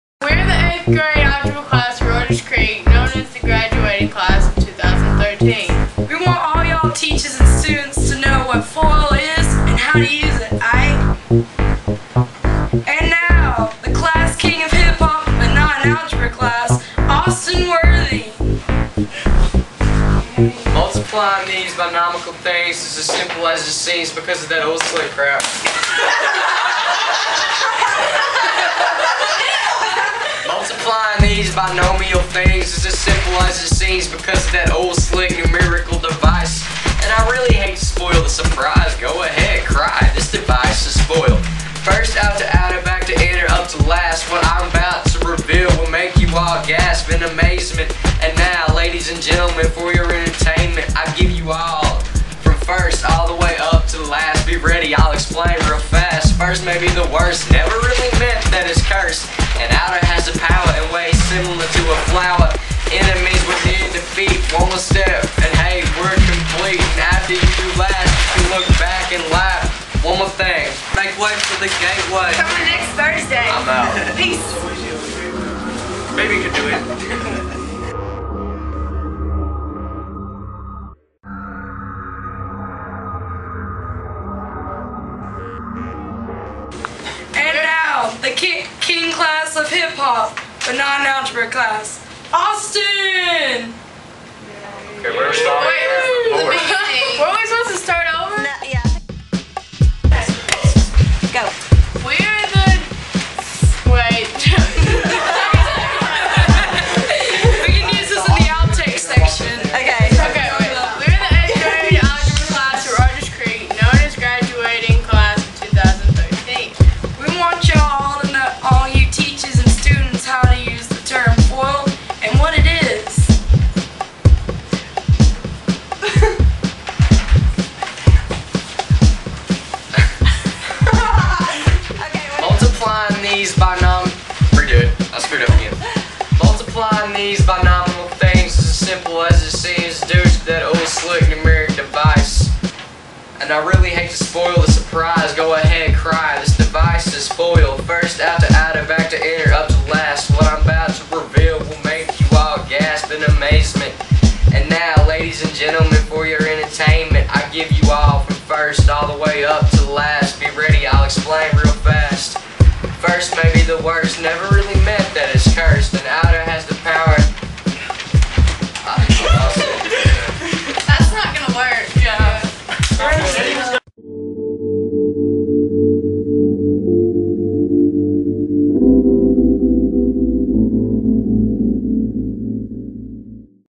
Multiplying Binomials
8th grade Algebra class rap about using the FOIL method for multiplying binomials.